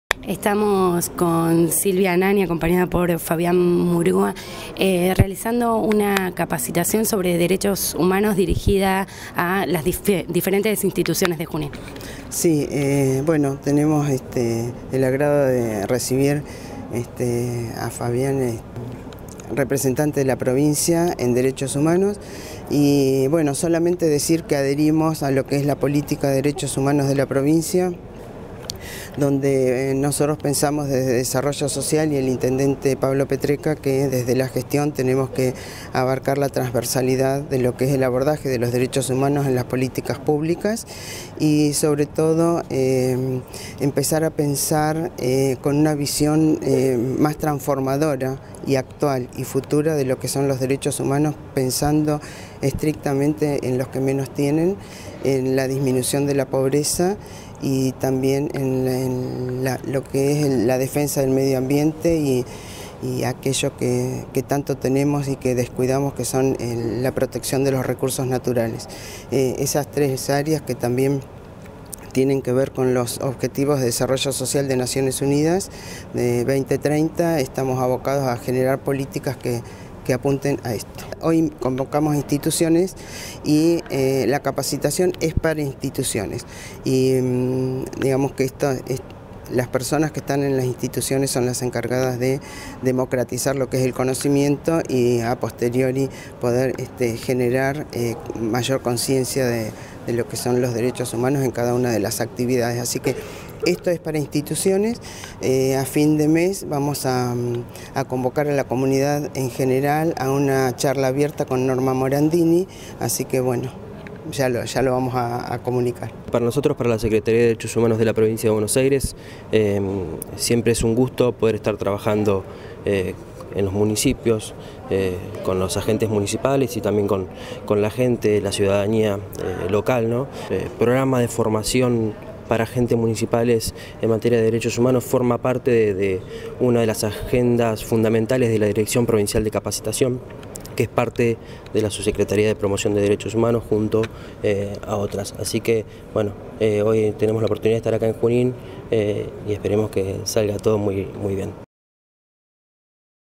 Importante capacitación sobre Derechos Humanos
Fue organizada por la secretaría de Desarrollo Social y Educación del Gobierno de Junín y la Secretaría de DDHH de la Provincia. Estuvo dirigida a referentes de distintas instituciones de la ciudad.